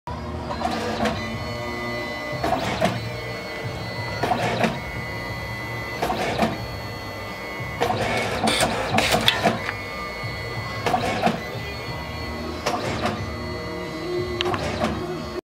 Instant Print Photo Booth
Allow your guests to capture the moment and instantly print their images whilst in the booth.